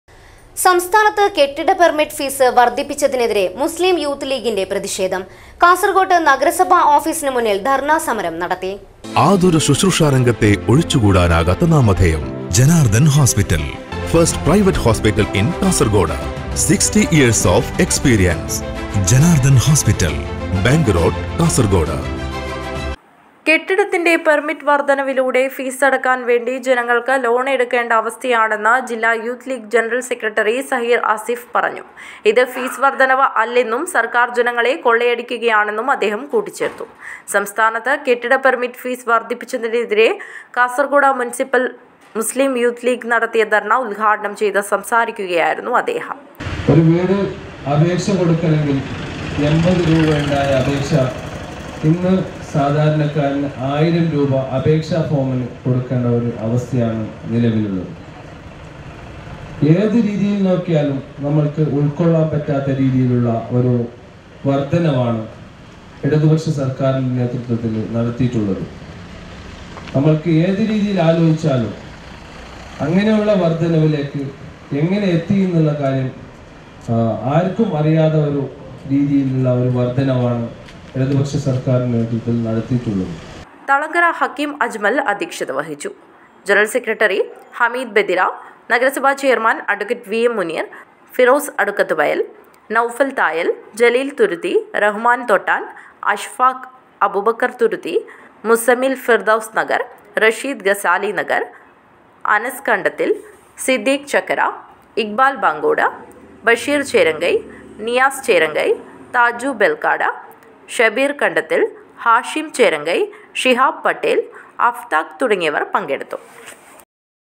സംസ്ഥാനത്ത് കെട്ടിട പെര്‍മിറ്റ് ഫീസ് വര്‍ധിപ്പിച്ചതിനെതിരെ മുസ്ലിം യൂത്ത് ലീഗിന്റെ പ്രതിഷേധം
സംസ്ഥാനത്ത് കെട്ടിട പെര്‍മിറ്റ് ഫീസ് വര്‍ധിപ്പിച്ചതിനെതിരെ മുസ്ലിം യൂത്ത് ലീഗിന്റെ പ്രതിഷേധം.  കാസര്‍കോട്ട്   നഗരസഭ ഓഫീസിനു മുന്നില്‍ ധര്‍ണ സമരം നടത്തി.